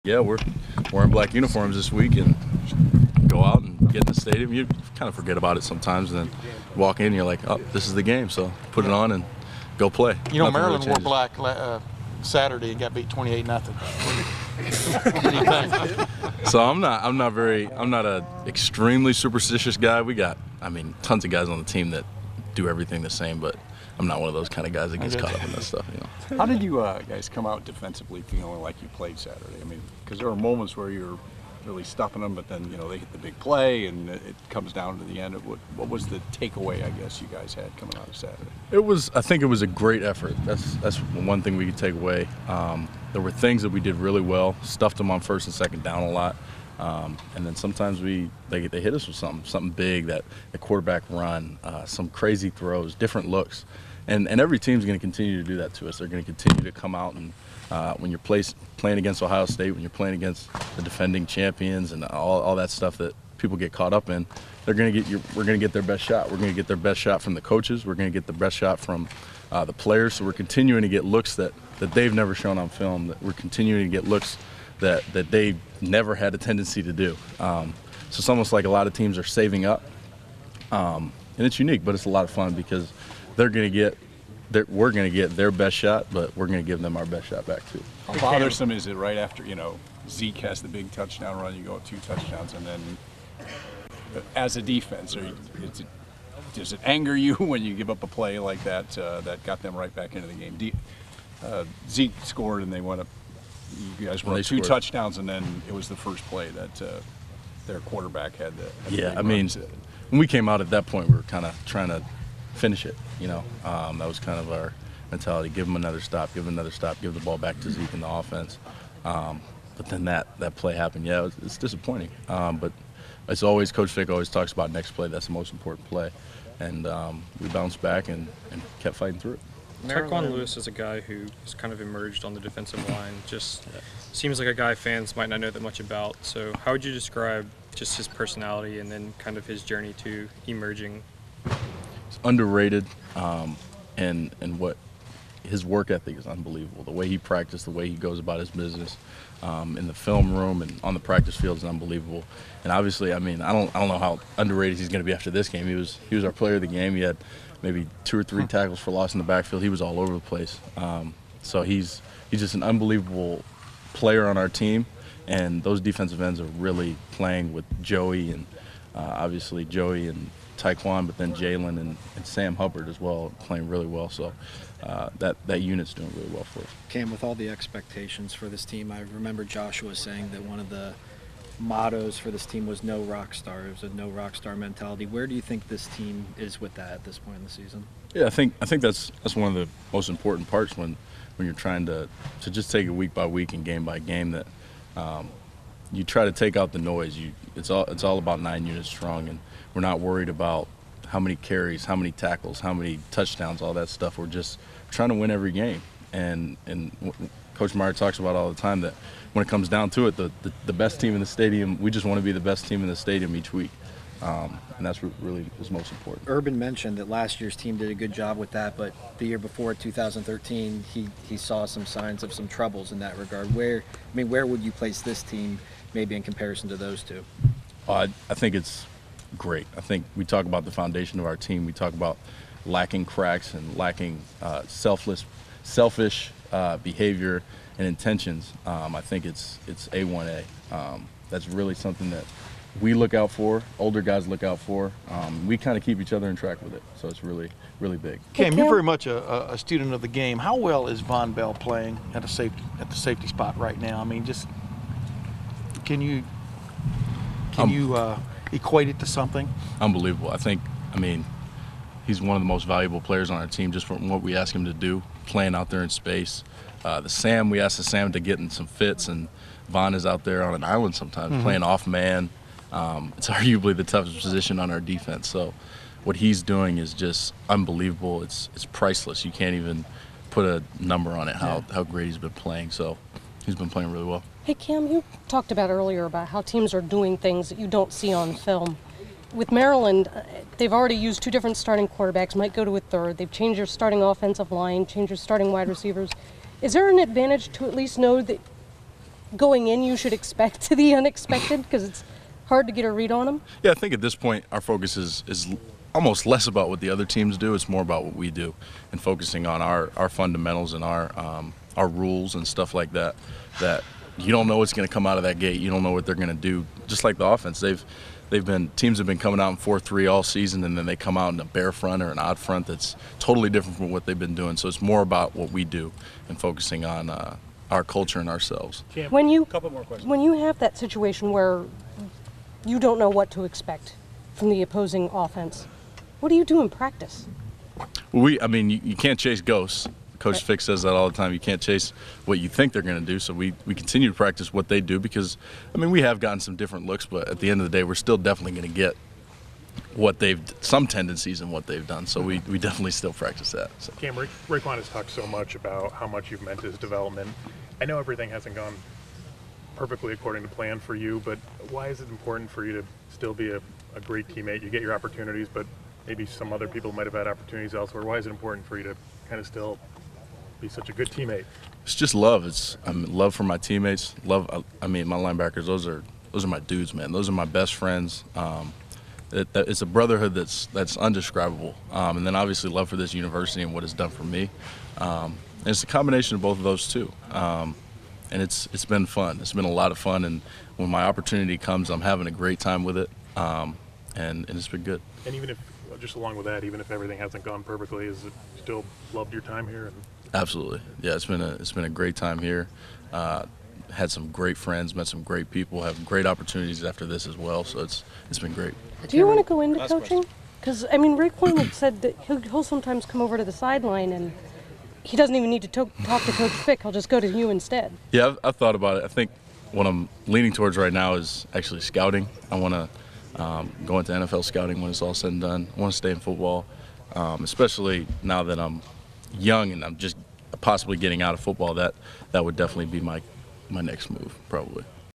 Ohio State LB Cam Williams 10/5/2015
Buckeyes Senior Linebacker Cam Williams on the black jerseys, the Defense and more as #1 OSU prepares for Maryland